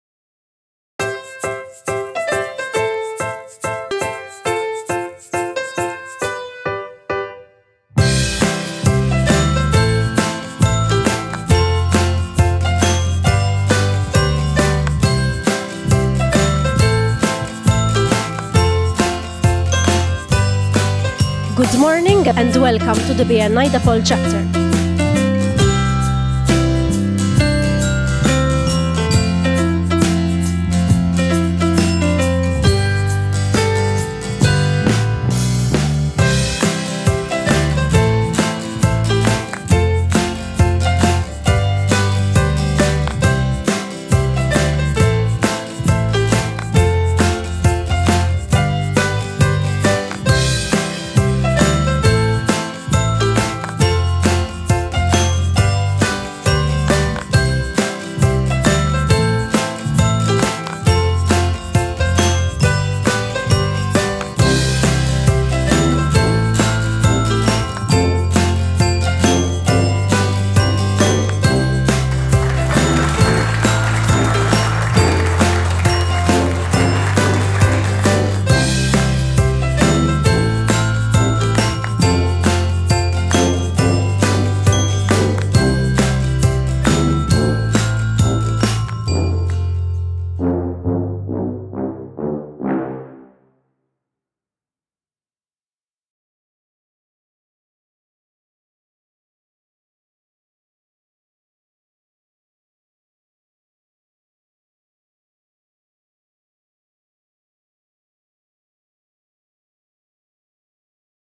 BNI De Paule Chapter, Attard, Malta
Are you ready for more Business?
BNI is the world's largest professional referral marketing organization.
Our chapter (BNI De Paule) is a dynamic, committed group of business people who know how to refer business to each other. This is a short video clip, created to give you an inside look at how BNI works.
We meet every Wednesday at 6:45am at the Corinthia Palace Hotel in Attard. In just one meeting you will get to know over 25 business professionals from various sectors. The positive energy generated during our meeting gives you a good start to your Wednesday mornings.
If you are looking to grow your business, visit BNI De Paule chapter and learn how BNI can help you.